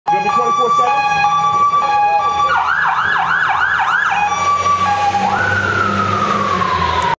Hook and Ladder Fire Truck runs through Cruise
This truck did 2 laps of Main Street at least? Not sure why it drove down the street with no lights on and then came back lights and sirens blaring as it ripped through the Cruise!